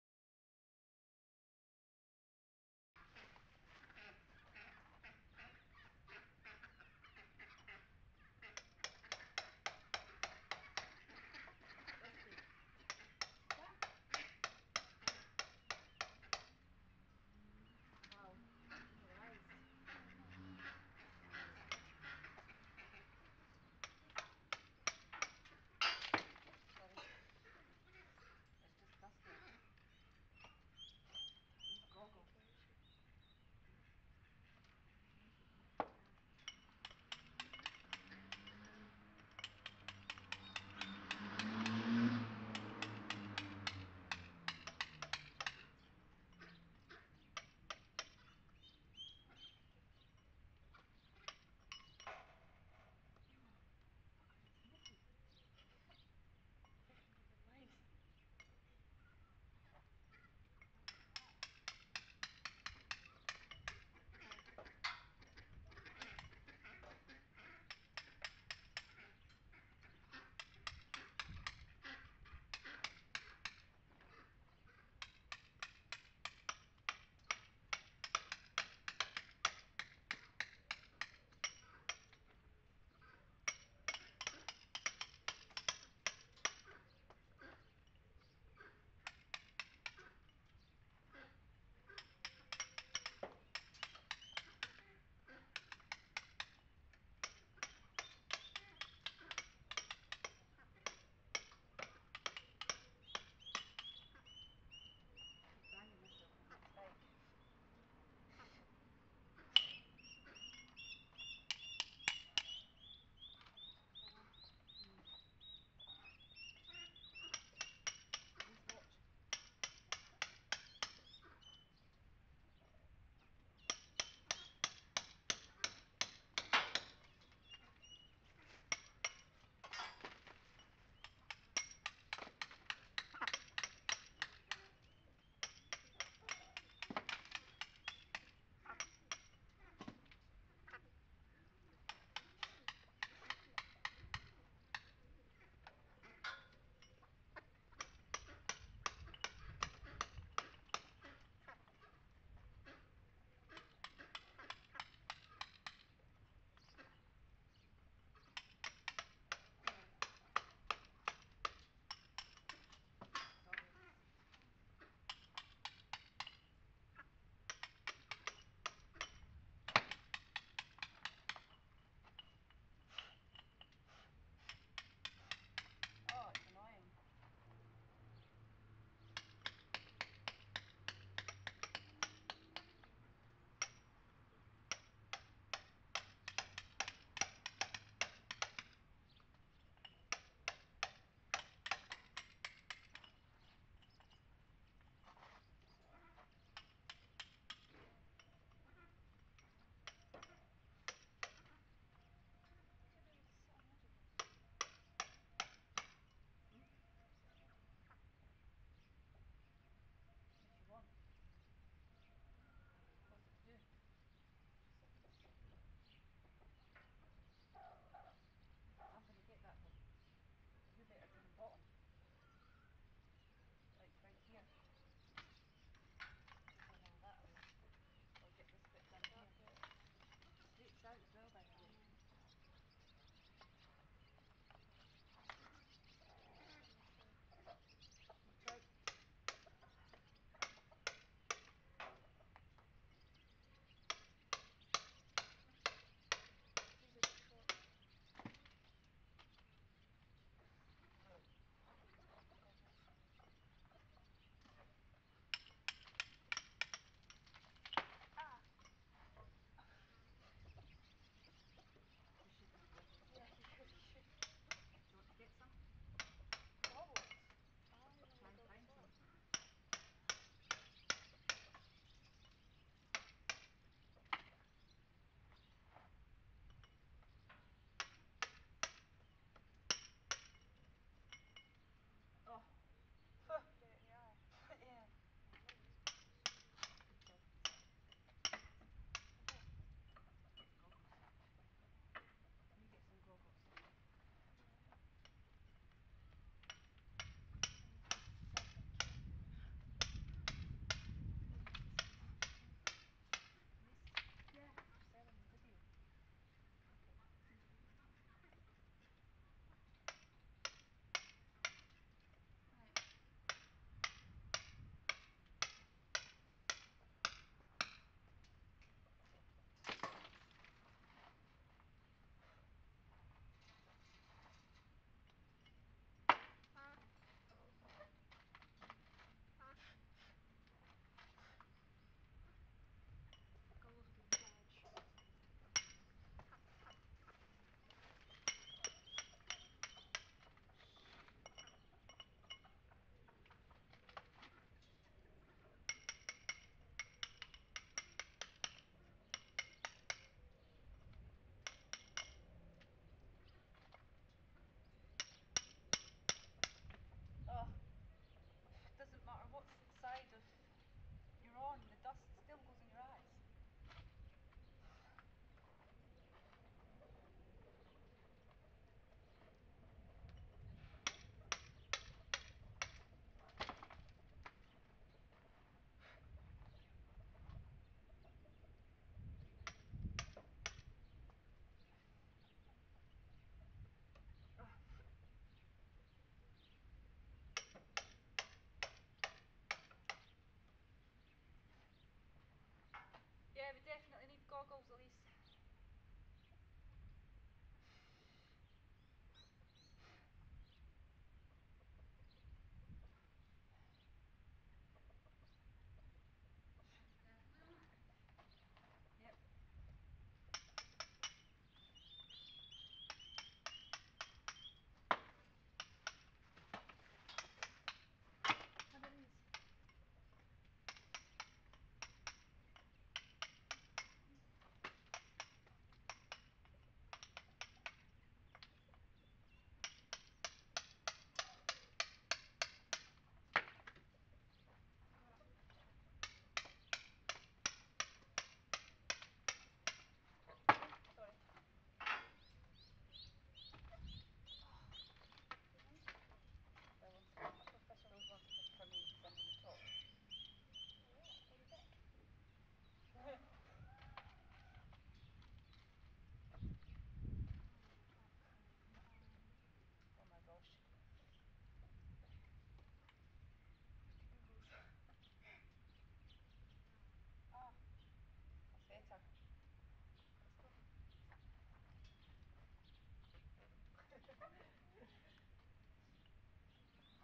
VLOG 1 Removing the old Mortar from the Old Forge (1784)
Chiselling the old forge before repointing